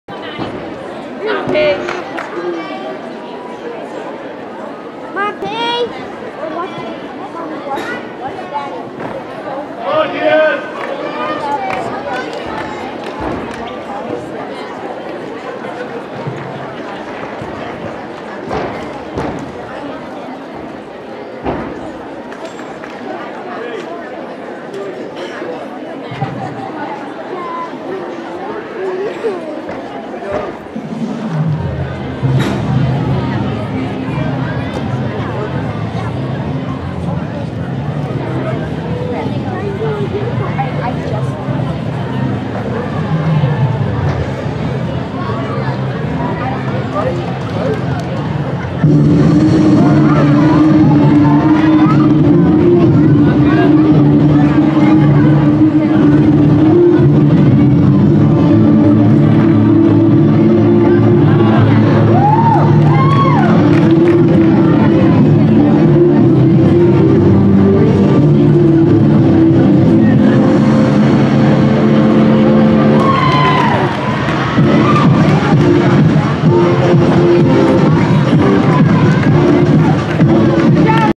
Brestyans HOPEs Compulsory 8.25 Floor Routine
My 1st ever HOPEs Floor Routine